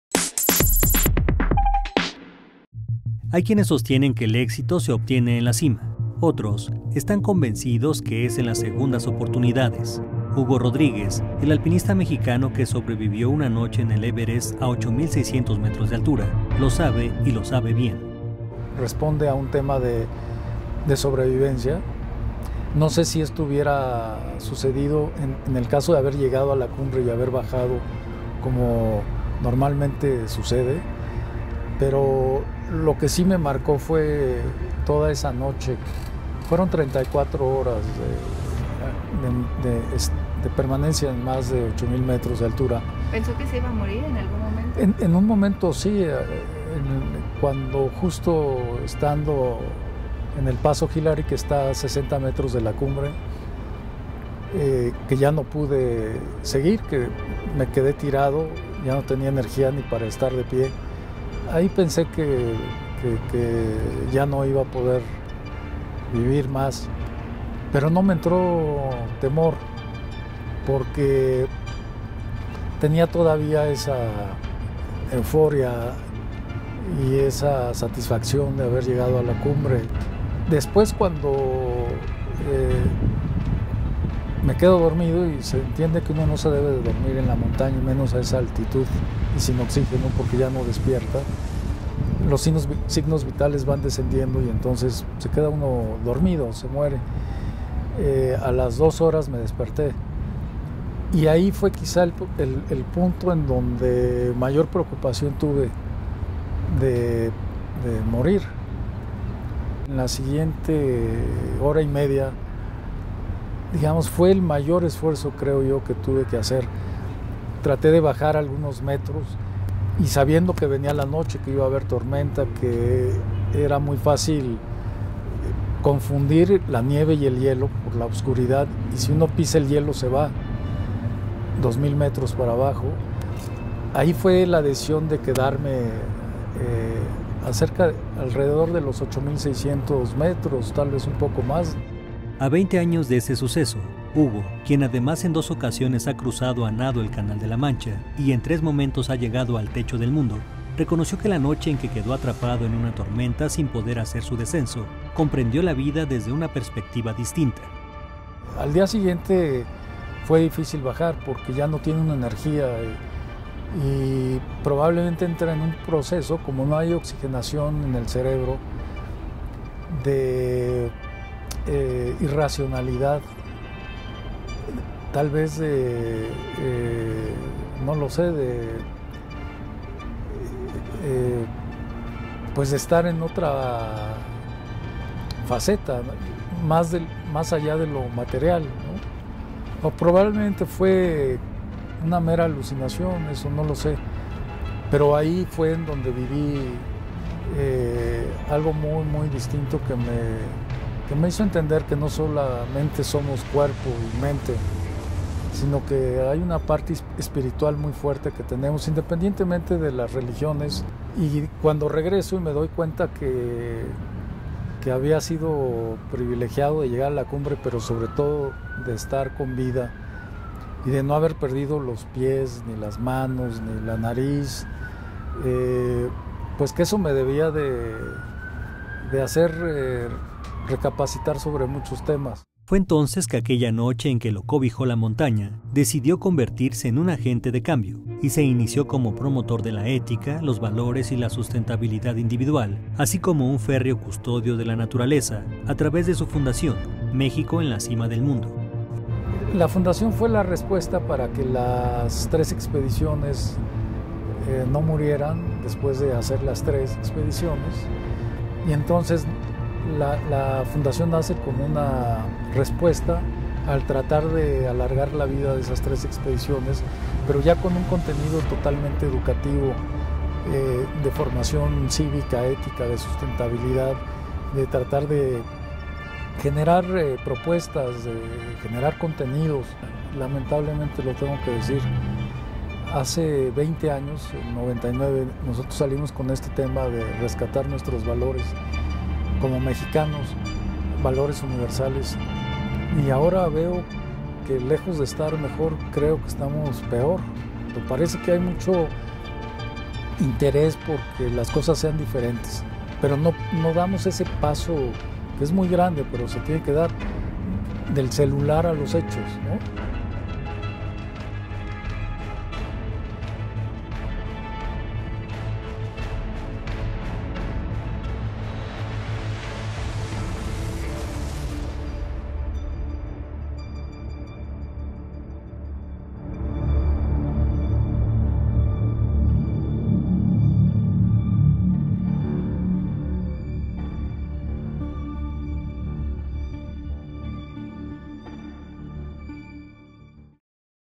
Hugo Rodríguez, de sobreviviente a promotor de valores
En 1997 Hugo Rodríguez llegó a la cima del Everest, pero al quedar atrapado en una tormenta se convirtió en sobreviviente al pasar una noche a 8 mil 600 metros de altura.